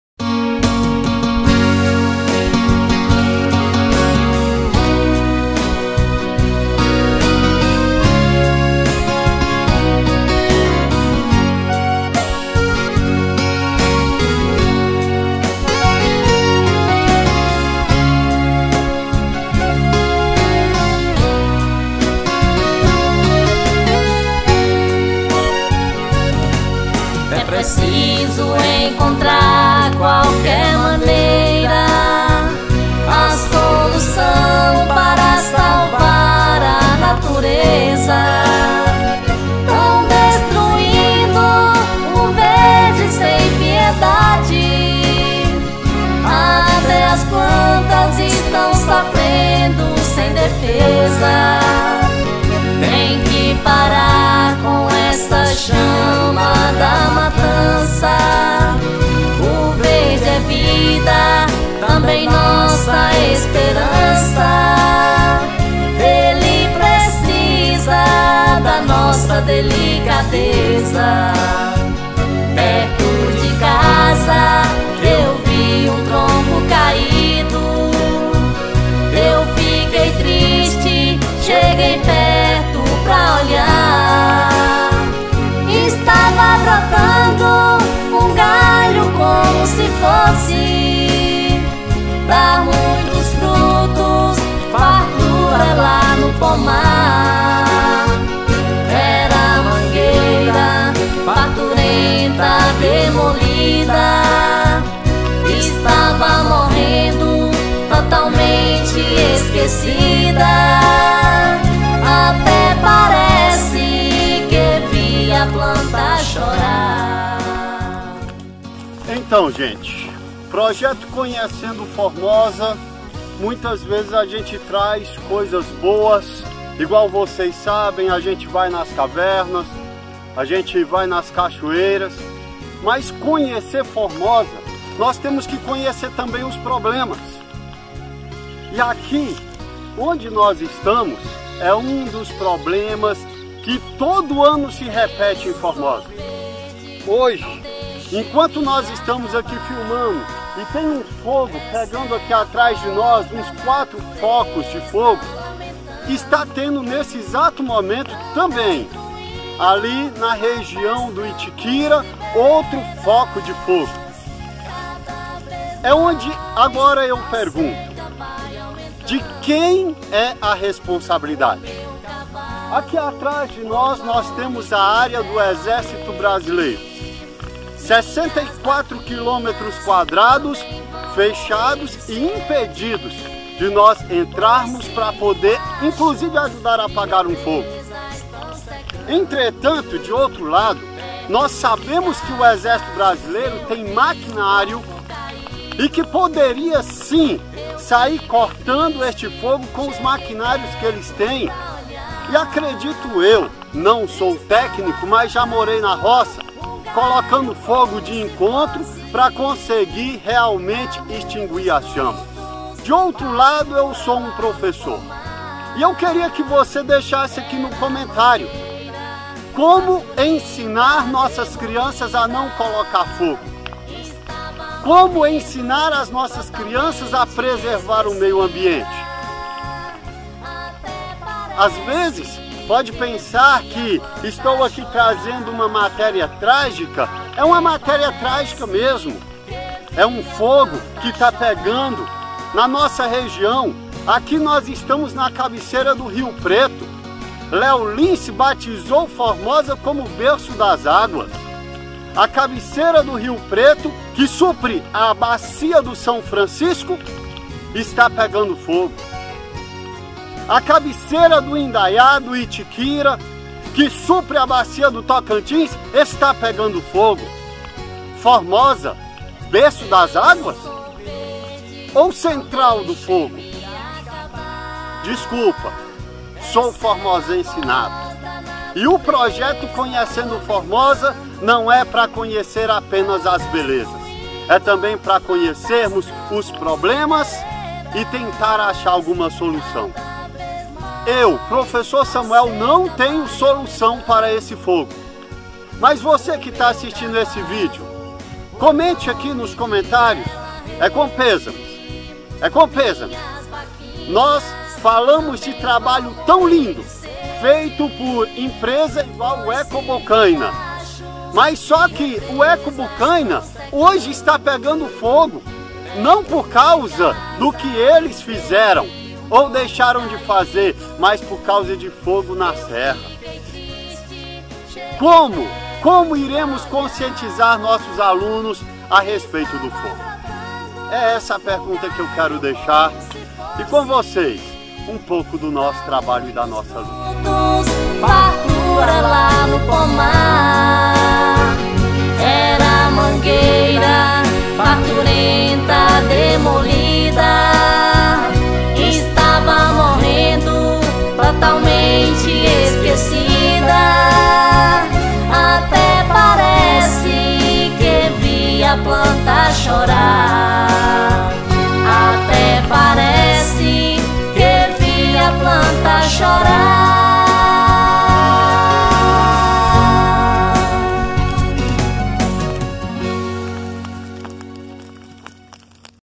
Fogo no Cerrado
O que falar sobre os focos de incêndio que tomam conta de nossa região todo ano???
Só dizer que realmente a umidade está muito baixa é o suficiente?
O que dizer para os nossos alunos???
Como conscientizar nossos compatriotas formosenses???
Como prevenir???
Qualquer solução que você perceber como viável, escreva aqui nos comentários...